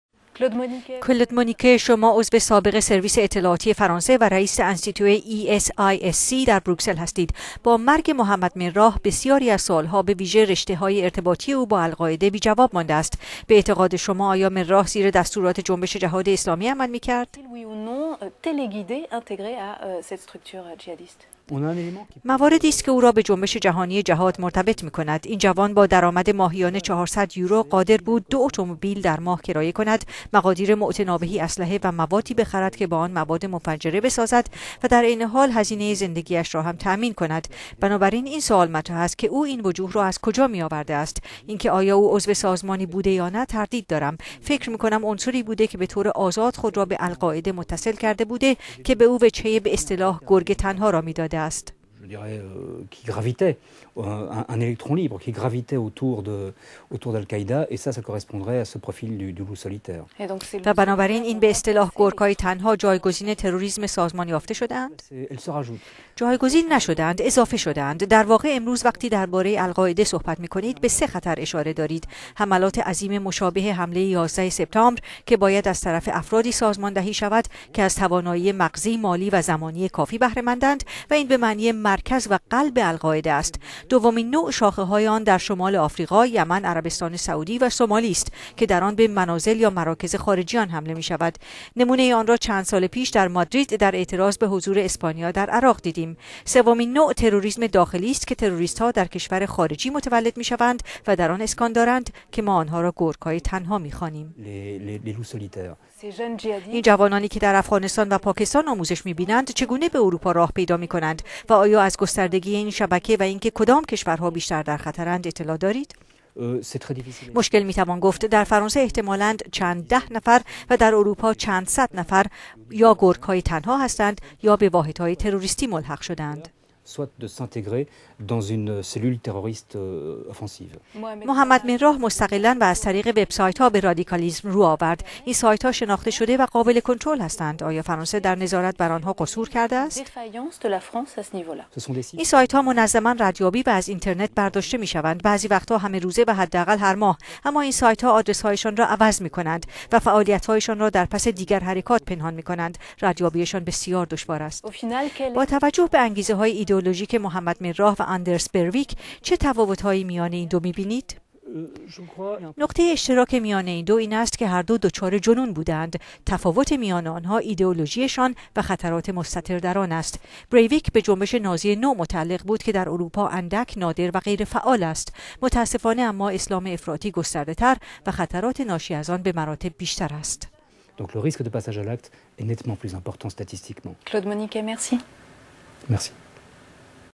گفتگو با کلود مونیکه، کارشناس تروریسم
یورونیوز: کلود مونیکه، شما عضو سابق سرویس اطلاعاتی فرانسه و رئیس انستیتوی ای اس آی اس سی در بروکسل هستید. با مرگ محمد مراح بسیاری از سئوالها بویژه رشته های ارتباطی او با القاعده، بی جواب مانده است. به اعتقاد شما، آیا مراح زیر دستورات جنبش جهاد اسلامی عمل می کرد؟

کلود مونیکه:"مواردی است که او را به جنبش جهانی جهاد مرتبط می کند: این جوان با درآمد ماهیانه چهارصد یورو  قادر بود دو اتوموبیل در ماه کرایه کند، مقادیر معتنابهی اسلحه و موادی بخرد که با آن مواد منفجره بسازد و در عین حال هزینه زندگیش را هم تامین کند. بنابراین، این سئوال مطرح است که او این وجوه را از کجا میاورده است.  اینکه آیا او عضو سازمانی بوده یا نه، تردید دارم. فکر می کنم عنصری بوده که بطور آزاد خود را به القاعده متصل کرده بوده که به او وجهه به اصطلاح گرگ تنها را می داده است".

یورونیوز: ...و بنابراین، این به اصطلاح گرگهای تنها جایگزین تروریسم سازمان یافته شده اند؟

کلود مونیکه:" جایگزین نشده اند، اضافه شده اند. در واقع، امروز وقتی درباره القاعده صحبت می کنید، به سه خطر اشاره دارید: حملات عظیم مشابه حمله یازده سپتامبر که باید از طرف افرادی سازماندهی شود که از توانایی مغزی، مالی و زمانی کافی بهره مندند...و این به معنی مرکز و قلب القاعده است. دومین نوع، شاخه های آن در شمال آفریقا، یمن، عربستان سعودی و سومالی است که در آن به منازل یا مراکز خارجیان حمله می شود . نمونه آن را چند سال پیش در مادرید در اعتراض به حضور اسپانیا در عراق دیدیم.  سومین نوع، تروریسم داخلی است که  تروریستها در کشور خارجی متولد می شوند و در آن اسکان دارند که ما آنها را گرگهای تنها می خوانیم".

یورونیوز:این جوانانی که در افغانستان و پاکستان آموزش می بینند، چگونه به اروپا راه پیدا می کنند و آیا از گستردگی این شبکه و اینکه کدام کشورها بیشتر در خطرند، اطلاع دارید؟

کلود مونیکه:"مشکل می توان گفت. در فرانسه احتمالا چند ده نفر و در اروپا چند صد نفر یا گرگهای تنها هستند یا به واحد های تروریستی ملحق شده اند."

یورونیوز: محمد مراح مستقلا و از طریق وبسایتها به رادیکالیسم رو آورد. این سایت ها شناخته شده و قابل کنترل هستند.  آیا فرانسه در نظارت بر آنها قصور کرده است؟

کلود مونیکه:"این سایت ها منظما ردیابی و از اینترنت برداشته می شوند،  بعضی وقتها همه روزه و حداقل هر ماه، اما این سایت ها آدرسهایشان را عوض می کنند، آدرس جدیدشان را در فوروم ها می دهند و فعالیتهایشان را در پس دیگر حرکات پنهان می کنند. ردیابیشان بسیار دشوار است."

یورونیوز: با توجه به انگیزه های ایدئولوژیک محمد مراح و آندرس بریویک، چه تفاوتهایی میان ایندو می بینید؟

کلود مونیکه:"نقطه اشتراک میان ایندو این است که هردو دچار جنون بوده اند. تفاوت میان آنها ایدئولوژیشان و خطرات مستتر در آن است. بریویک به جنبش نازی نو متعلق بود که در اروپا اندک، نادر و غیر فعال است. متاسفانه، اما، اسلام افراطی گسترده تر و خطرات ناشی از آن بمراتب بیشتر است".